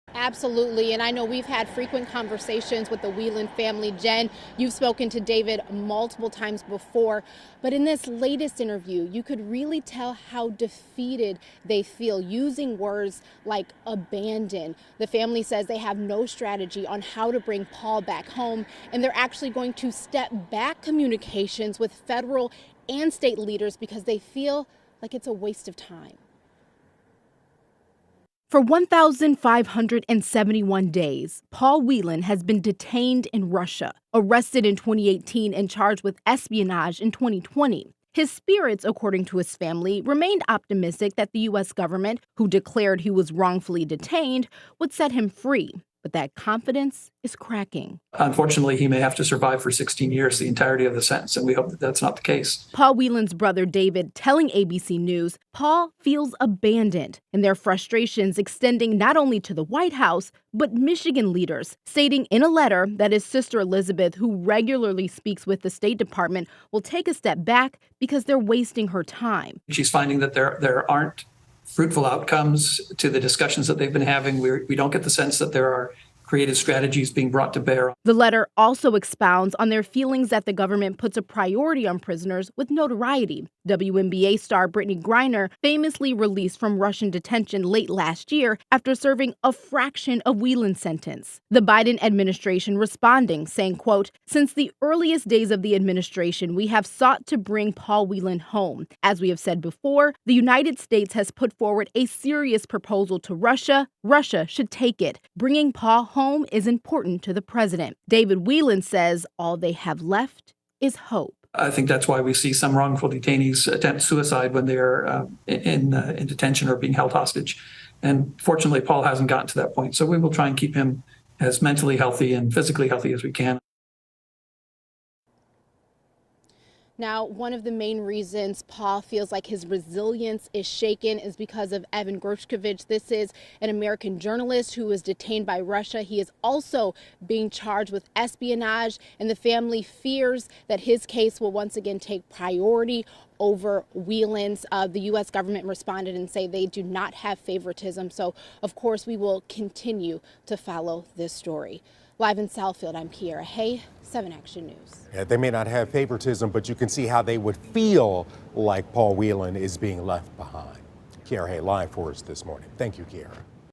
Paul Whelan says he feels 'abandoned' by the United States
For 1,571 days, Paul Whelan of Novi has been detained in Russia. He was arrested in 2018 and charged with espionage in 2020.  His family says his spirits were at first high as he was optimistic that the U.S. declared him wrongfully detained and would set him free.   But that confidence is dwindling as Paul still waits for freedom behind bars.  “Unfortunately he may have to survive 16 years the entirety of his sentence, but we hope that’s not the case,” David Whelan said.